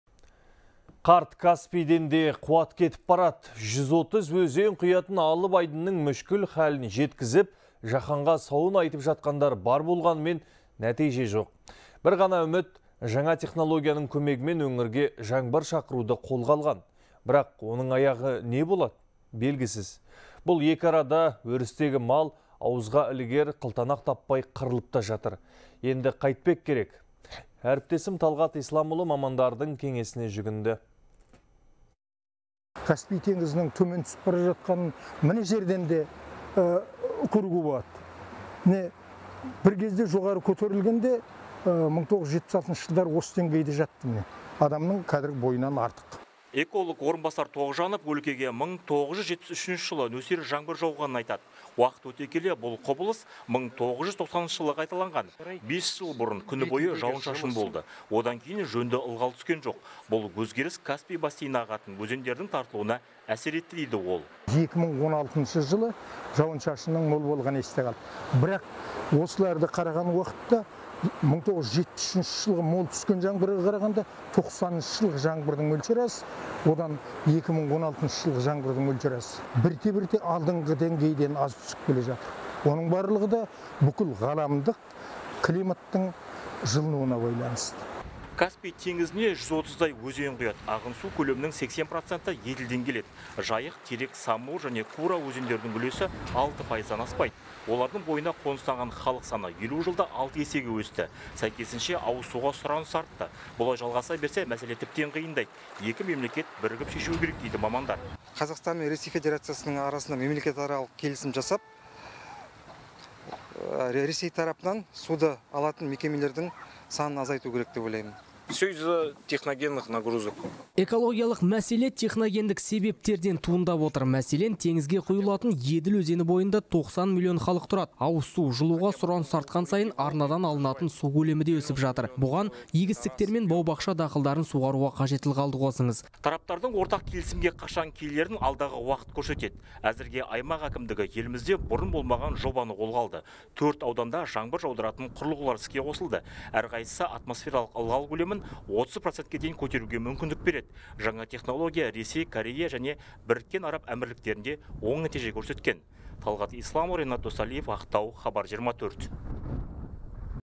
Каспий теңізі тартылып барады
Қарт Каспийден де қуат кетіп барады. 130 өзен құятын алып айдынның мүшкіл халін жеткізіп, жаһанға сауын айтып жатқандар бар болғанмен, нәтиже аз. Бір ғана үміт – жаңа технологияның көмегімен өңірге жаңбыр шақыруды қолға алған. Бірақ оның аяғы не болады? Белгісіз. Бұл екіарада өрістегі мал ауызға ілігер қылтанақ таппай қырылып та жатыр.